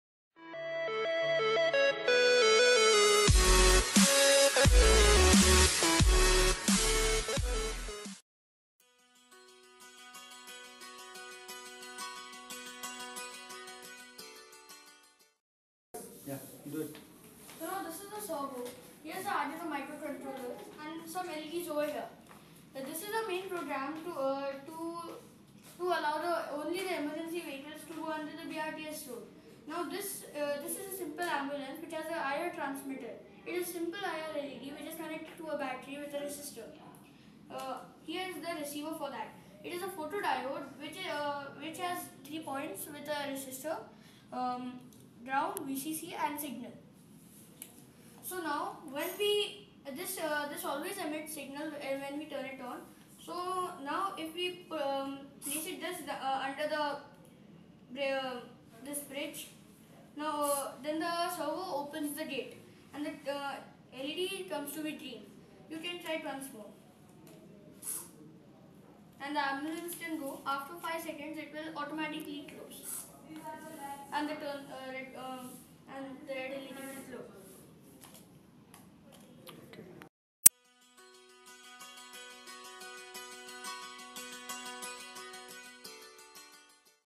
BRT Pune project
Model of BRT proiject in Pune. Enetering in BRT lane is allowed only for valid vehicles.